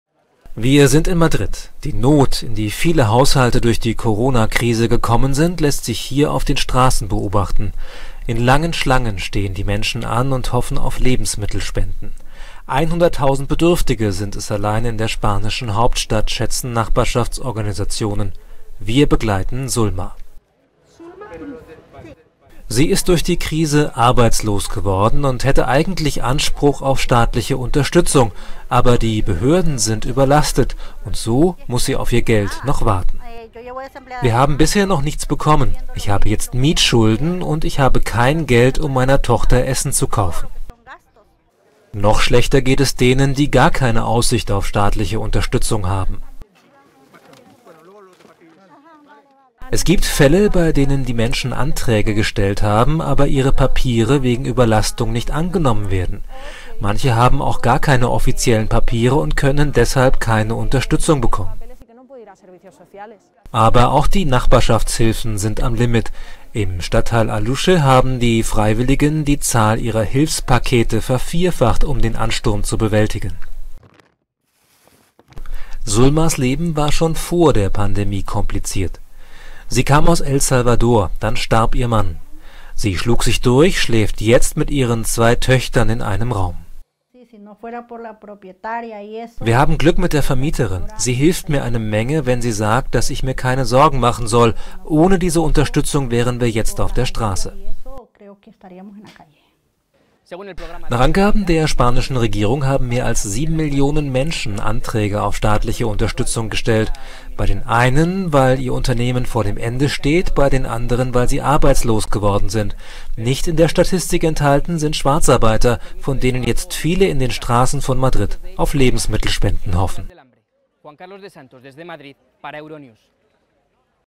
Alleinerziehend, arbeitslos, angewiesen auf Lebensmittelspenden in Madrid
Alleinerziehend, zwei Kinder, seit Corona arbeitslos. Für Sulma aus Madrid ist dieser Albtraum leider Wirklichkeit geworden. Wir haben sie begleitet.…